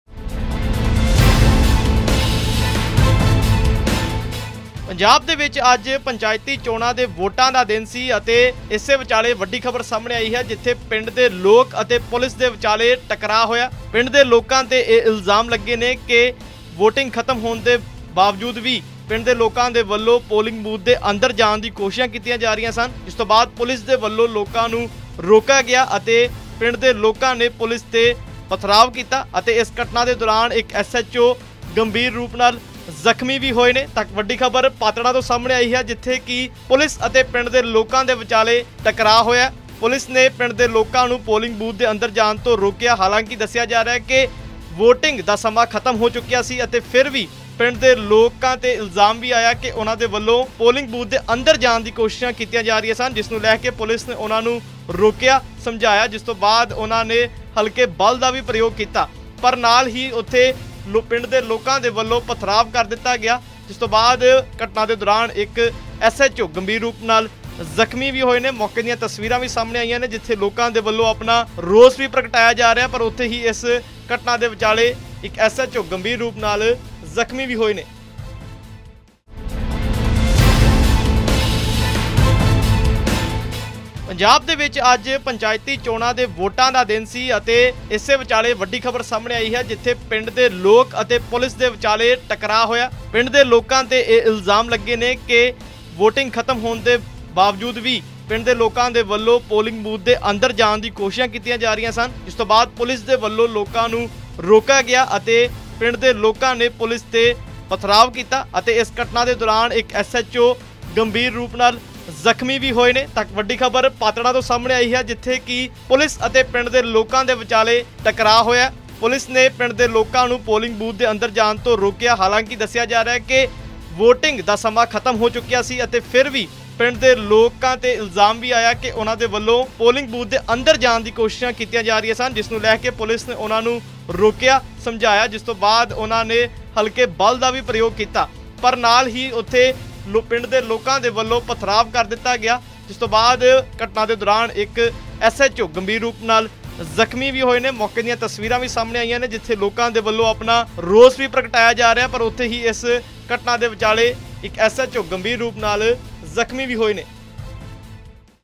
Patran News | ਬੂਥ ਦੇ ਬਾਹਰ ਹੀ ਪੈ ਗਿਆ ਗਾਹ, ਝੜਪ ਦੌਰਾਨ SHO ਹੋਇਆ ਜਖ਼ਮੀ| Clash | Panchayat Elections | N18V
ਪੁਲਿਸ ਤੇ ਲੋਕਾਂ 'ਚ ਟਕਰਾਅ, ਝੜਪ ਦੌਰਾਨ SHO ਹੋਇਆ ਜਖ਼ਮੀ। ਪਿੰਡ ਵਾਲਿਆਂ ਨੇ ਕੀਤੀ ਨਾਅਰੇਬਾਜ਼ੀ, ਬੂਥ ਦੇ ਬਾਹਰ ਹੀ ਪੈ ਗਿਆ ਗਾਹ।